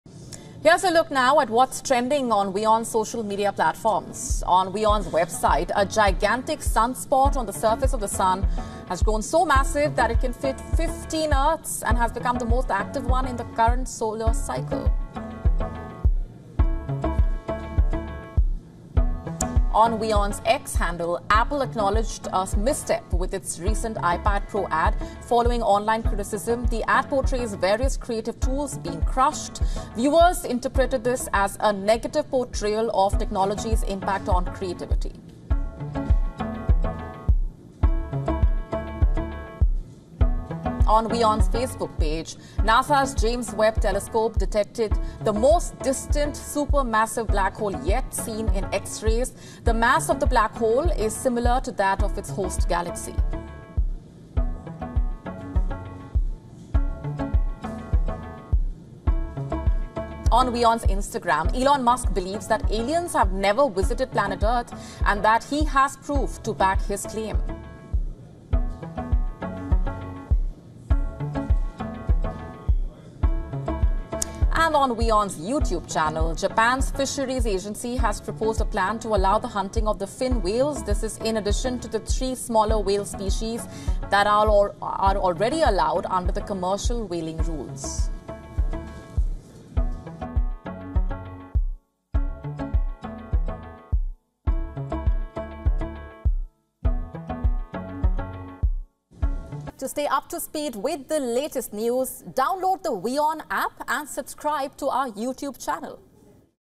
Gigantic sunspot wide enough to fill 15 worlds becomes visible | Trending on WION
Here, we bring you the news that is trending across WION's social media platforms:
- Gigantic sunspot wide enough to fill 15 worlds becomes visible
- Apple apologises for its controversial iPad pro ad of creative tools being crushed
- NASA detects the farthest supermassive black hole ever observed
- Elon Musk says aliens have never visited Earth